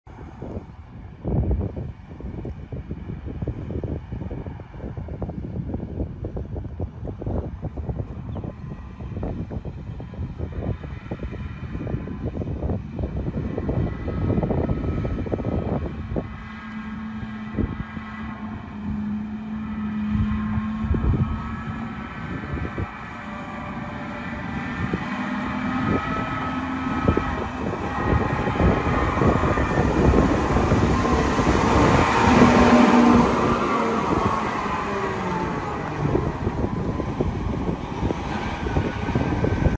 2018 CATERPILLAR 982M For Sale
2018 with 8,334 hours, 4 New Tires 875/65R29 L-4 Maxam's, 10 Yard GP Bucket, Payload Scale, Auto Lube
Freshly serviced, Thru the shop, Resealed both tilt cylinders, Ready to go to work.